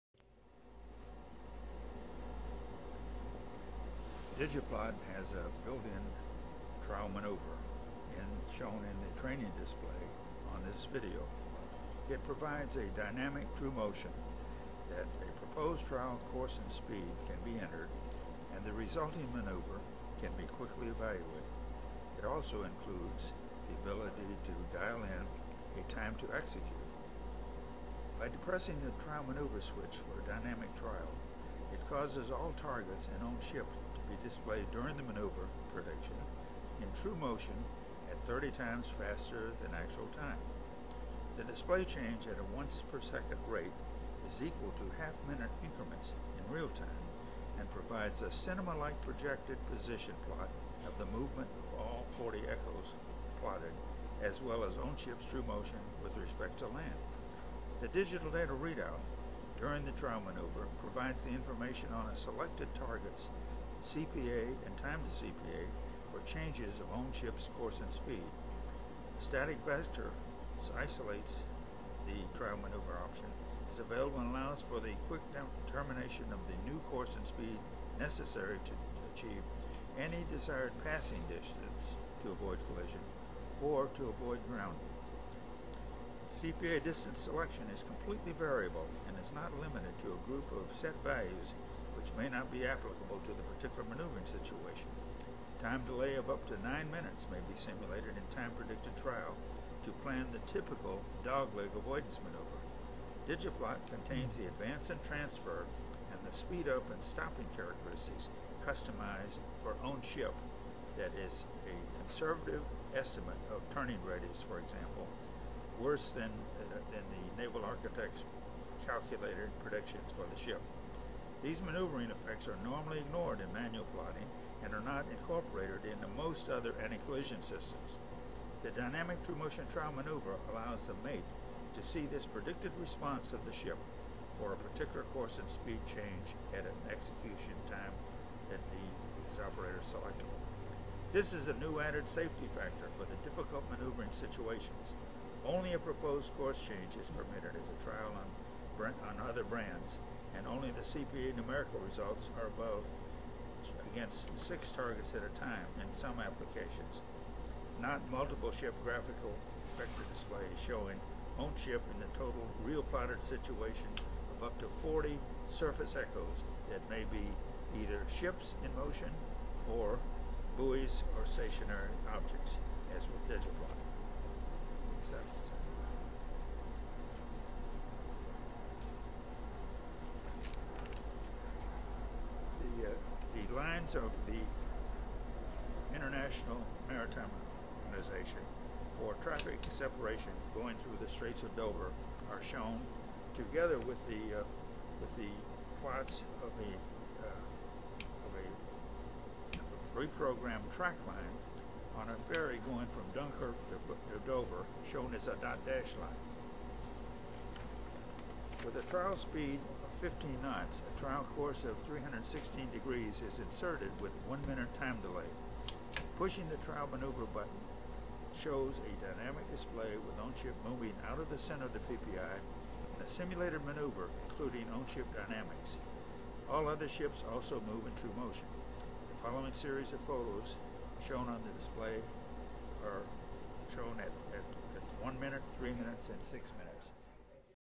Digiplot Trainer with Trial Maneuver Ship Dynamics
ARPA Anti-collision and grounding prevention using dynamic trial maneuver including ships turning radius as a function of speed and ship size can cause deck officers without sufficient 'feel of the ship'